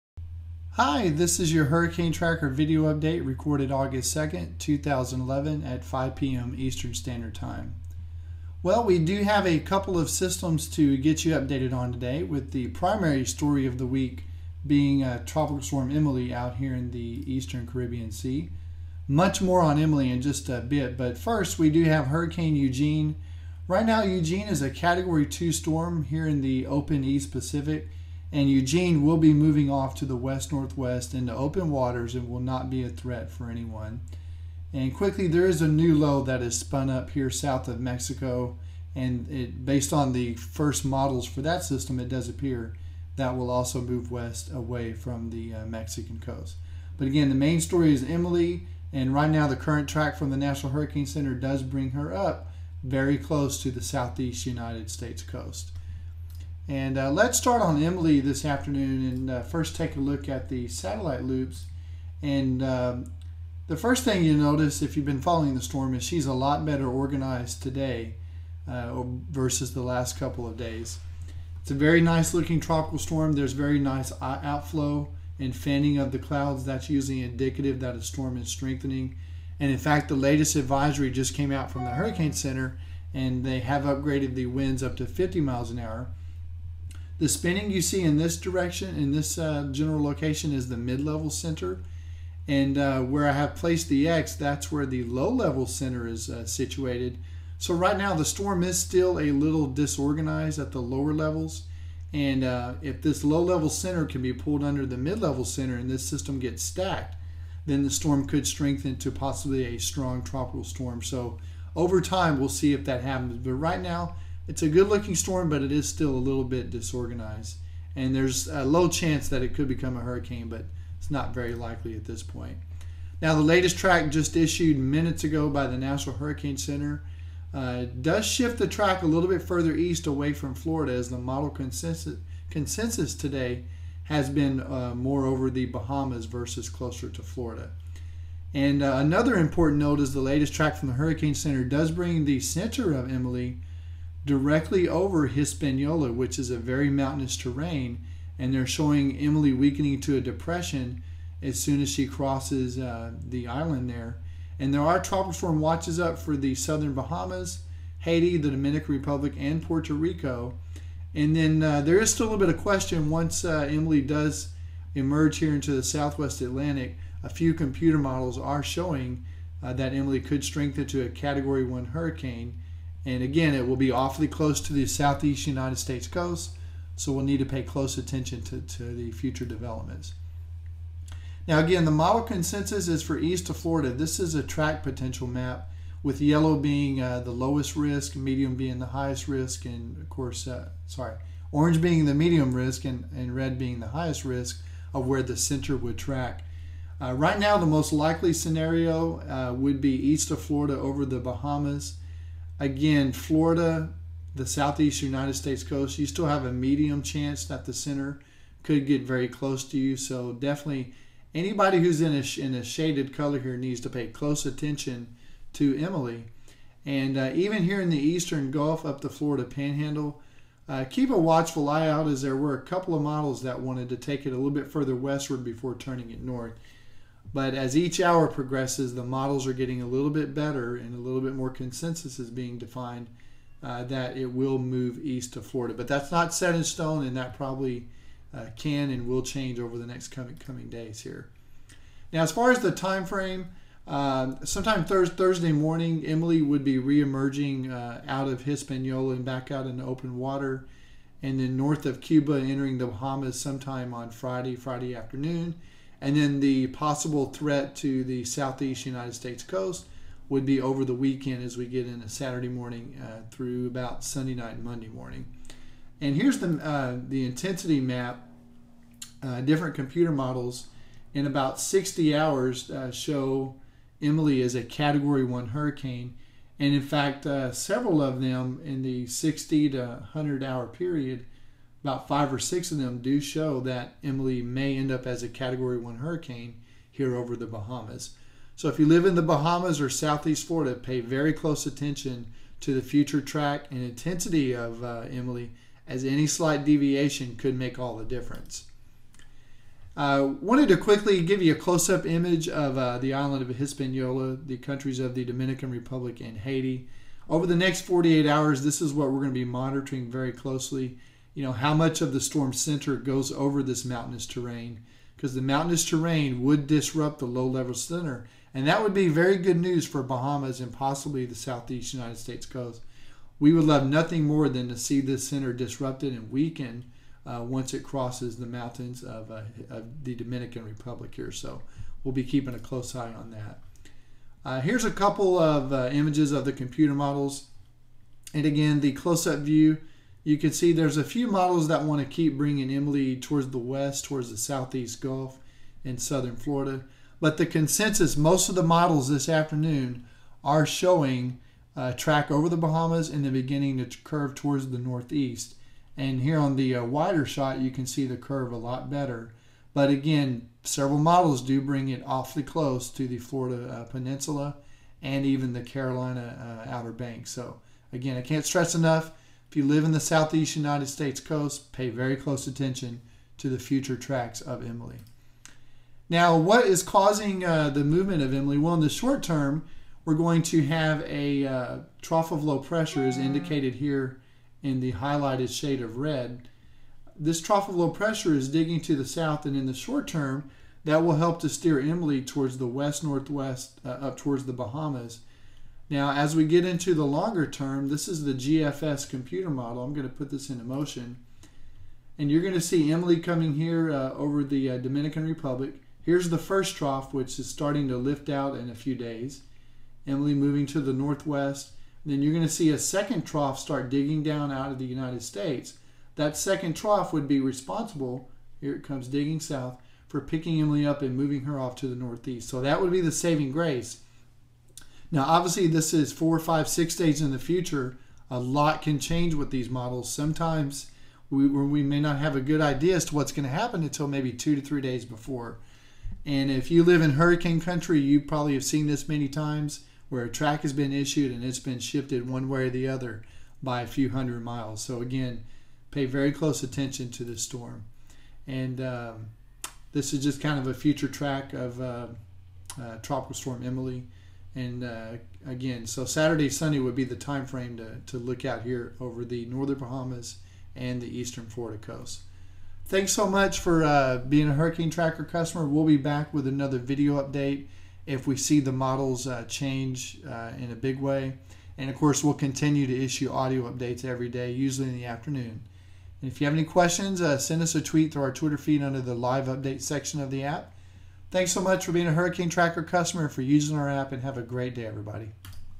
TS Emily Update
TS Emily is making a run in the Caribbean Sea and towards the SE US coast. How close will she get to the US and what are the steering factors.

You can find the complete Hurricane Tracker app in the iTunes app store simply by searching "Hurricane Tracker"